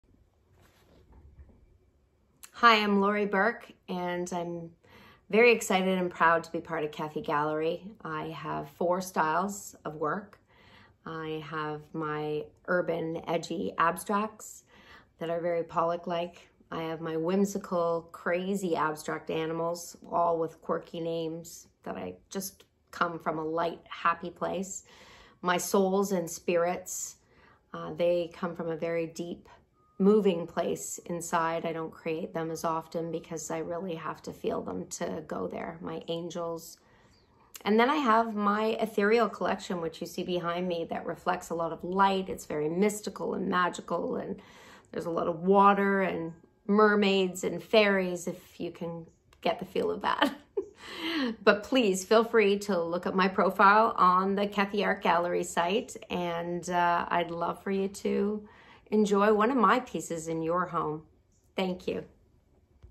Lori Burke | Artist Interview | Kefi Art Gallery
Lori Burke is a full-time Canadian artist.

As a self-taught intuitive artist, Lori Burke creates art that touches the heart and inspires the creative spirit. She adopted a spontaneous, bold, and fearless painting technique - a process of discovery - one that results in lush and colorful artworks. Lori responds to the moment with an openness to change, a variety of mediums, and expanses of color. Lori’s authentic self-expression evolved as she practices. She has multiple collections, some are dynamic and bold, while others are more calm and spiritual. Nevertheless, all of her works will bring tranquility and magic to any space, as well, will light up any room, and fill it with happiness. Intuition is the essence of the whole creative process. If art isn’t about feelings, then what is its purpose?